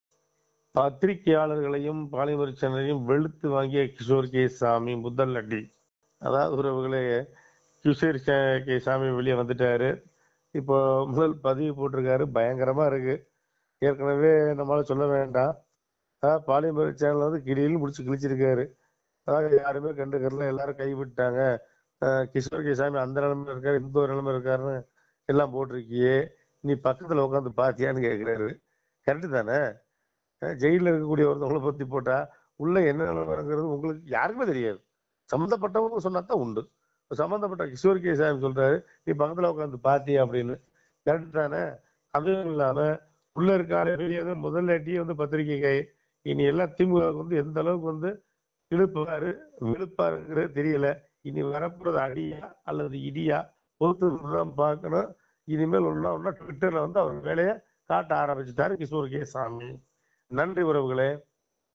ஆட்டம் ஆரம்பம் பத்திரிகையாளர்களையும் பாலிமரையும் வெளுத்து வாங்கிய கிஷோர் கே ஸ்வாமி!
ஆட்டம் ஆரம்பம் பத்திரிகையாளர்களையும் பாலிமரையும் வெளுத்து வாங்கிய கிஷோர் கே ஸ்வாமி!Kishorekswamy